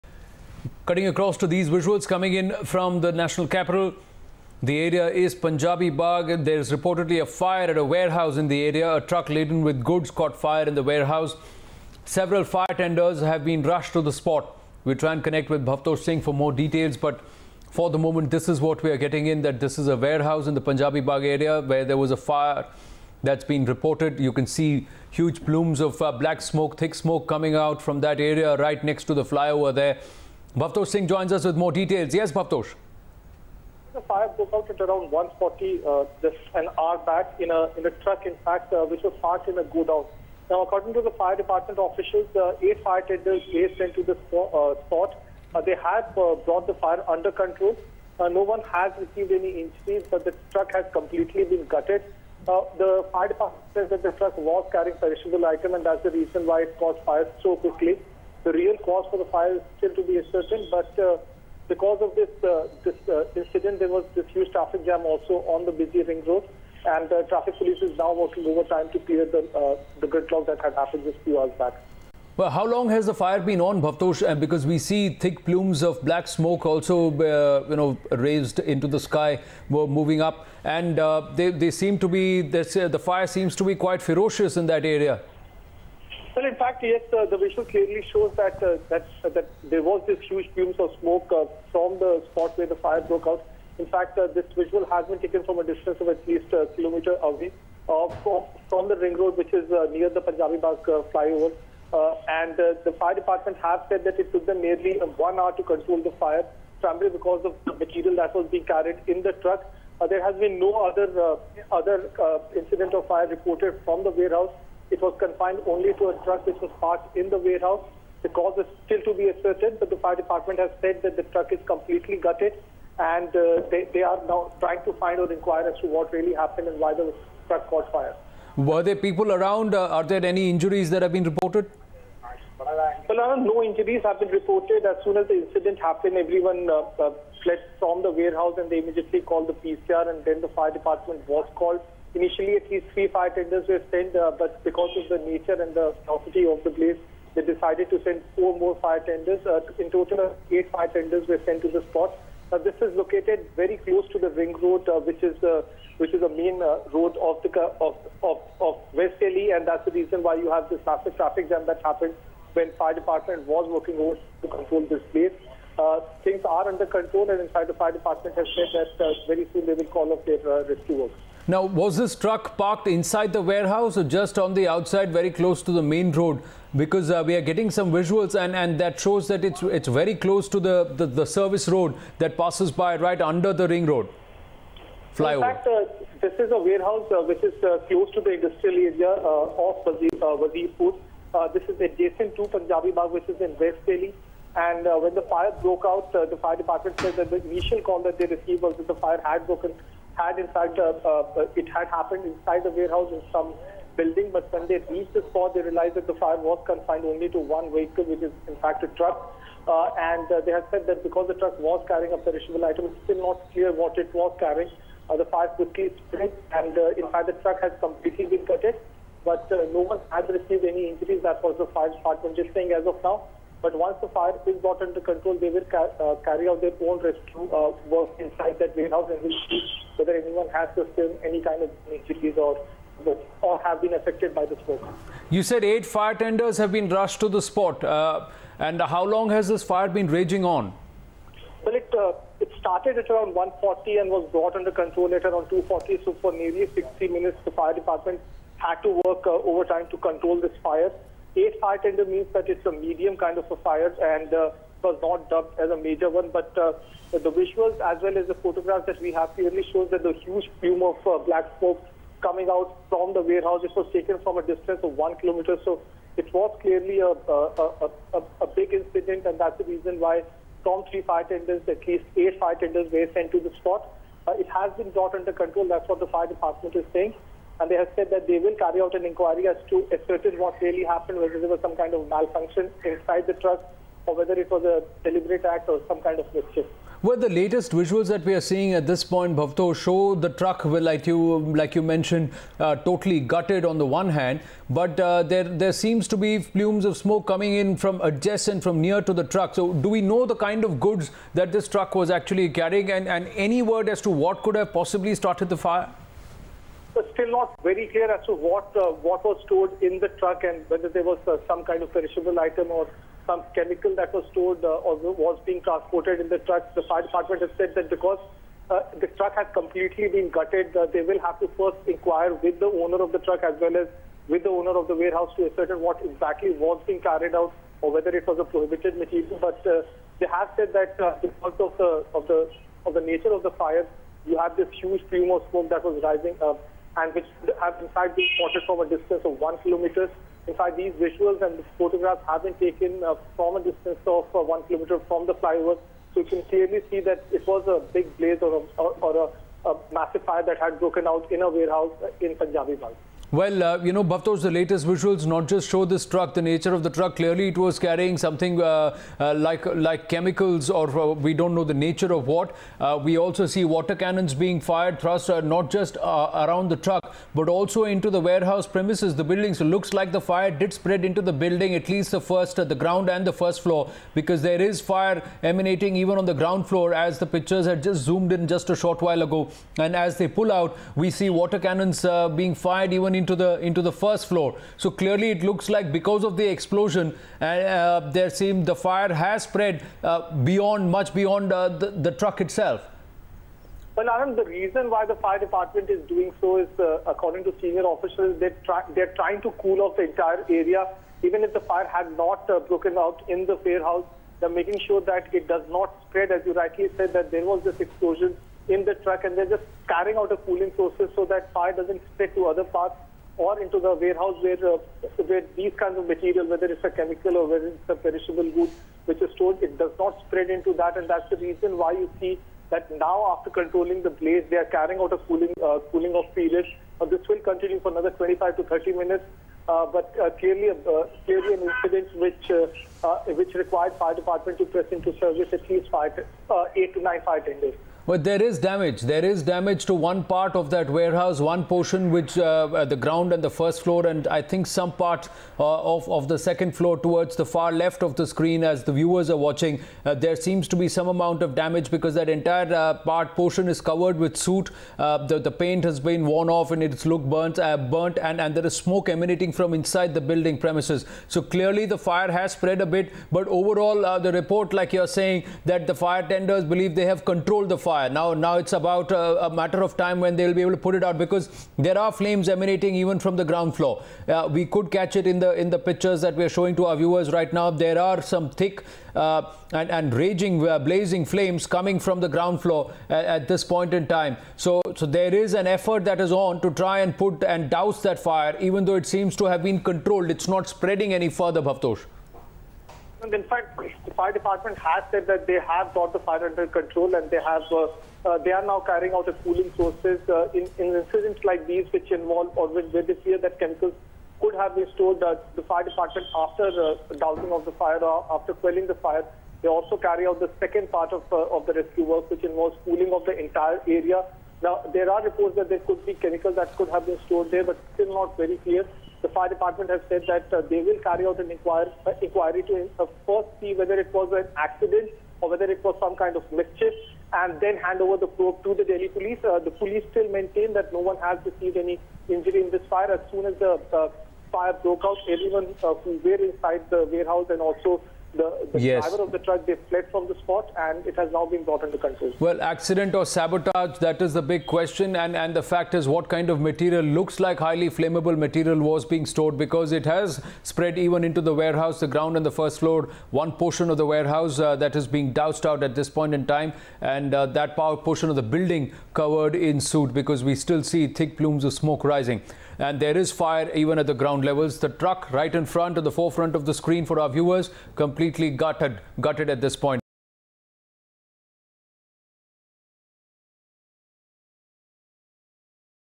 Fire at a warehouse in Punjabi Bagh
Fire broke out at a warehouse in Punjabi Bagh. A truck carrying goods caught fire in the warehouse. Several fire tenders have rushed to the spot.